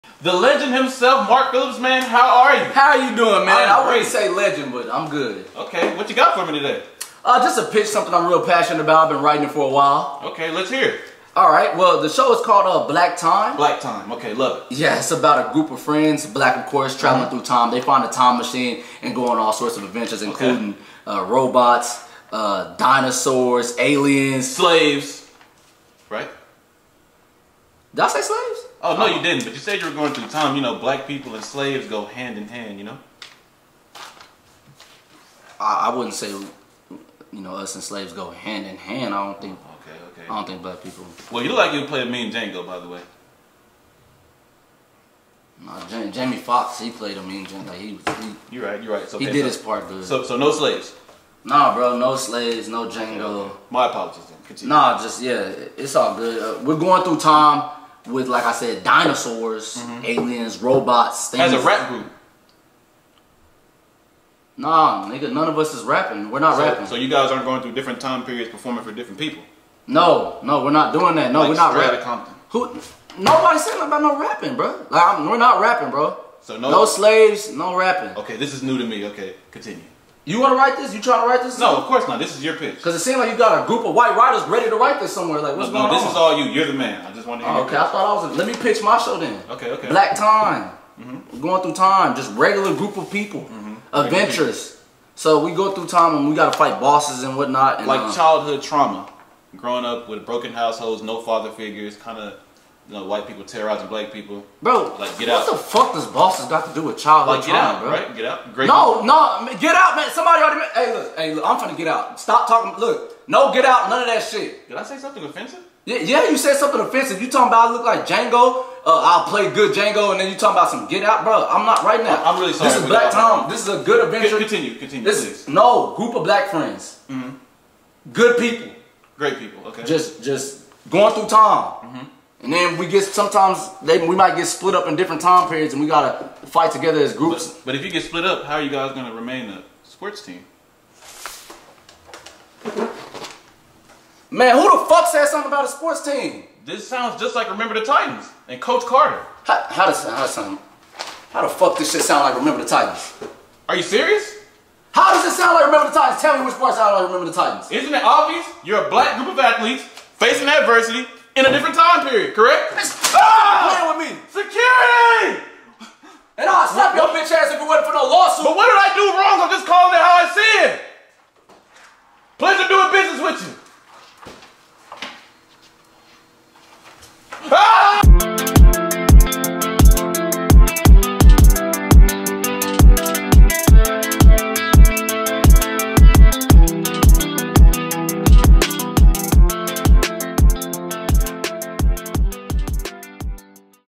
How Networks be when you Black and Try to Pitch a Show
😂😂😂😂 why every black show gotta have certain things in them bro let us make what we want!!

Mark Phillips - @SupremeDreams_1
Affiong Harris - @CleanUniform

DREAM CON INFO!